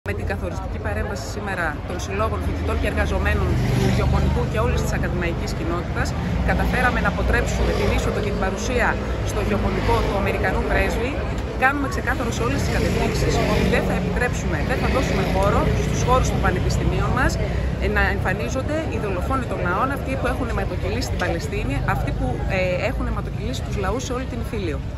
Κινητοποίηση στο Γεωπονικό ενάντια στην παρουσία του πρέσβη των ΗΠΑ
Από το πρωί ο Σύλλογος Εργαζομένων και ο Φοιτητικός Σύλλογος του Γεωπονικού Πανεπιστημίου βρέθηκαν στις πύλες του Ιδρύματος, καταγγέλλοντας τα σχέδια για την παρουσία του Αμερικανού πρέσβη στη σχολή, στα πλαίσια εκδήλωσης.

Κάτω από την καθοριστική παρέμβαση και καταδίκη των εργαζομένων και των φοιτητών, ο πρέσβης δεν εμφανίστηκε στο Ίδρυμα.